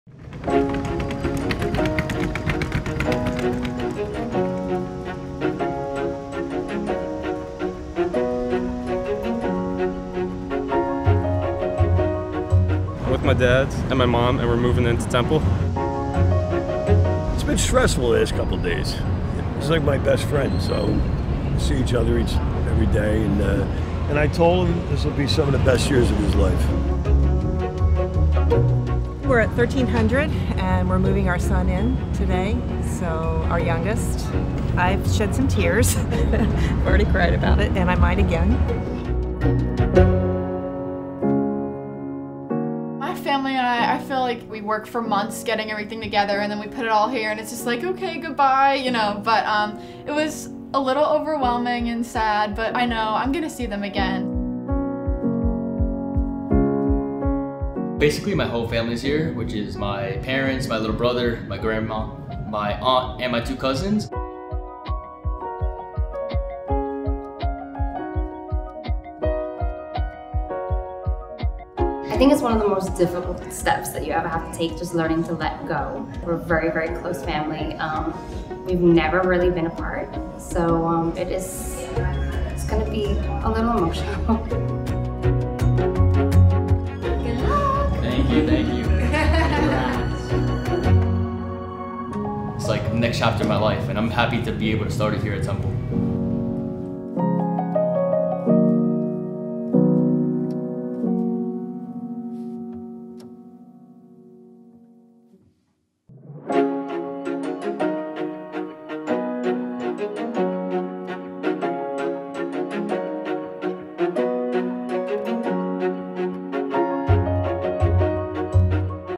Move-In Day at Temple University
Hear from some students and their families as they settle in to their new home on Temple's main campus in Philadelphia, Pennsylvania.